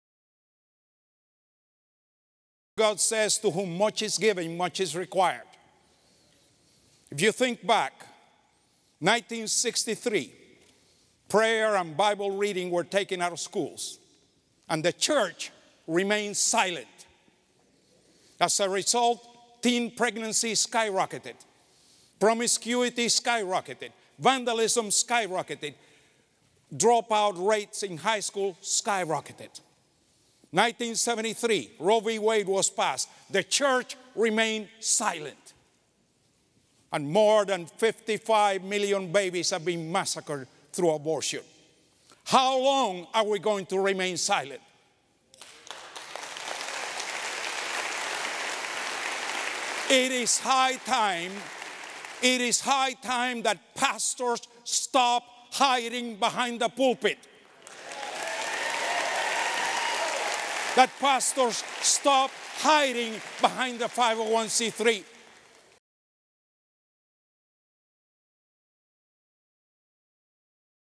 Pastor Rafael Cruz - A Short Message for Pastors...
Pastor Rafael Cruz has a short message for every pastor to hear. Has your pastor heard this?